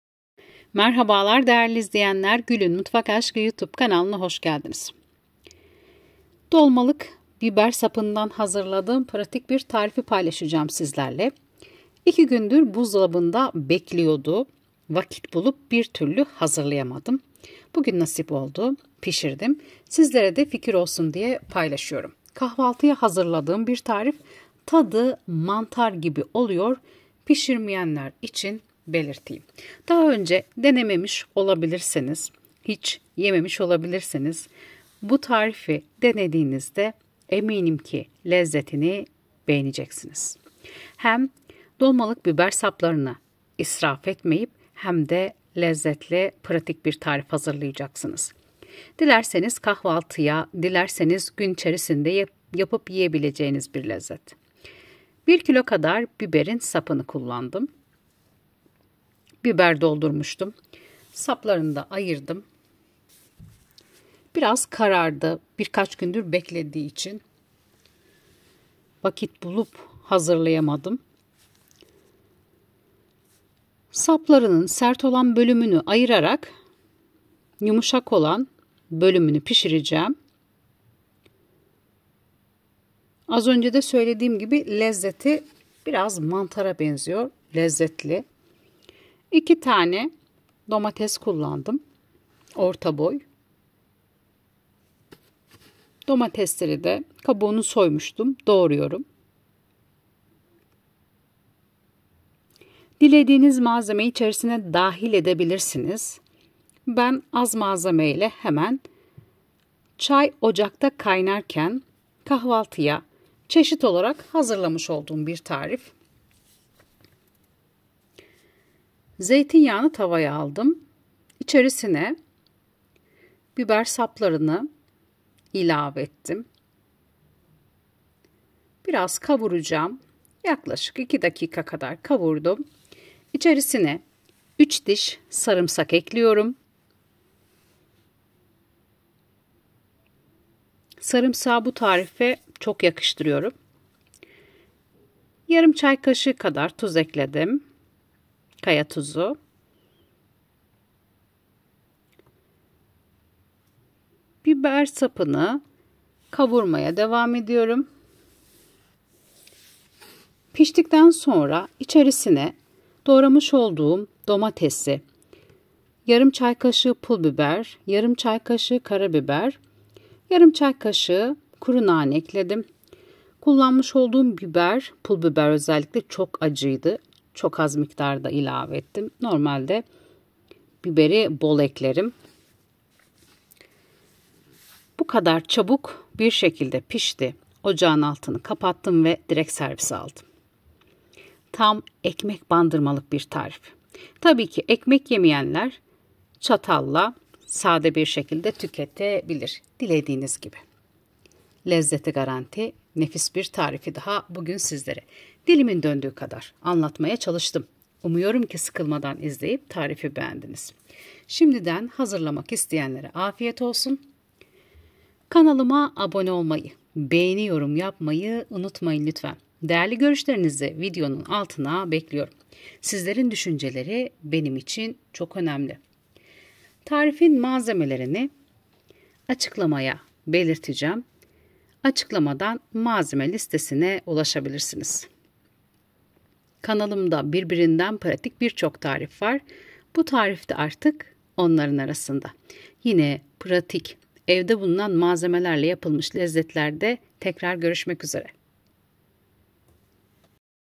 ANNEM NE ZAMAN BİBER DOLDURSA SAPLARINDAN HARİKA BİR TARİF YAPARDI İŞTE O PRATİK TARİFLERDEN BİRİSİ
1: kilo biberin sapı

1: Kahve fincanı zeytinyağı

2: Tane orta boy domates

3: Diş sarımsak

1: Yarım çay kaşığı kaya tuz